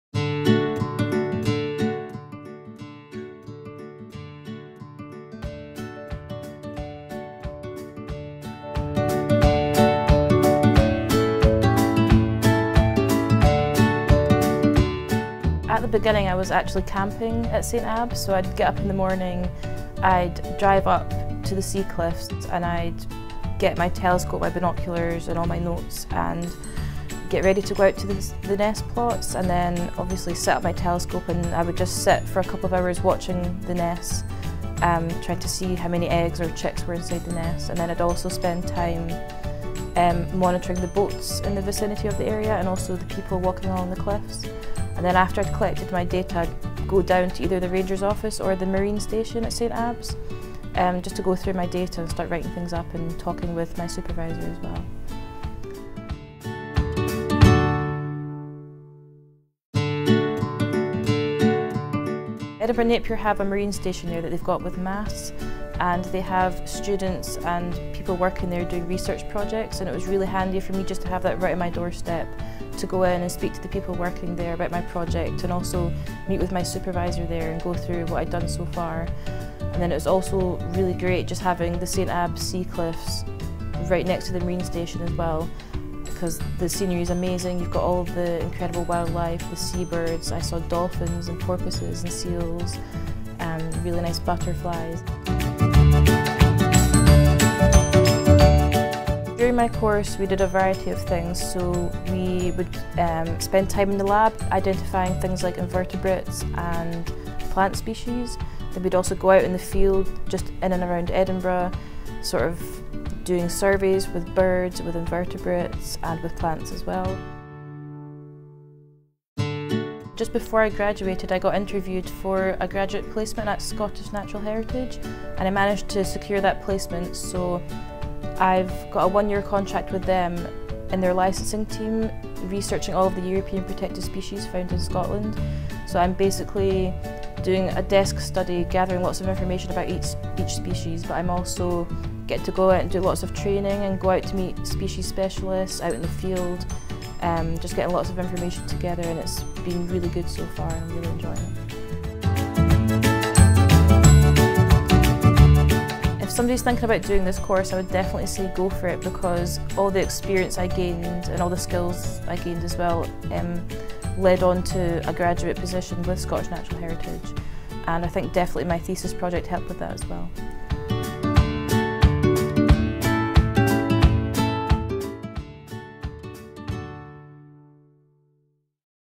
Edinburgh Napier University | SAS | MSc Graduate Jenna: Working with Scottish Natural Heritage
MSc Wilflife Conservation and Biology graduate Jenna tells us about camping out at St. Abbs fishing village during her masters thesis, the marine station at St. Abbs, and her graduate job at Scottish Natural Heritage. Find out more about the St. Abbs Marine Station which is co-directed by Edinburgh Napier University